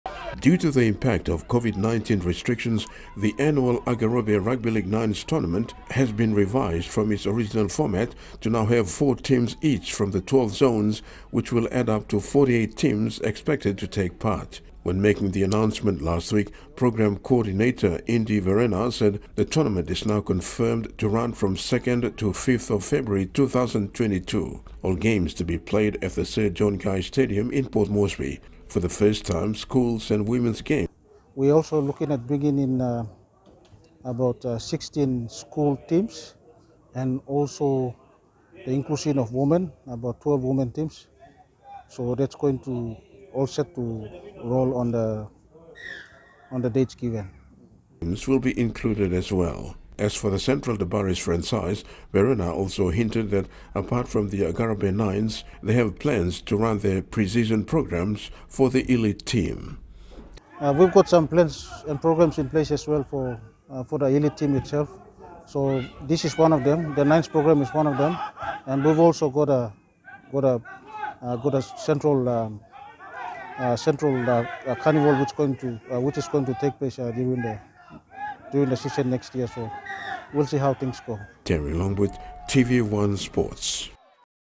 Agarobe Cup Feb
The Agarobe Nines Tournament is now reset for February 2022 with amendments to its original concept and venues.

According to the revised format, four teams from each of the 12 zones will participate at a central venue which will be the Sir John Guise stadium.

However according to the revised tournament program, it will involve schools and women’s rugby league as well.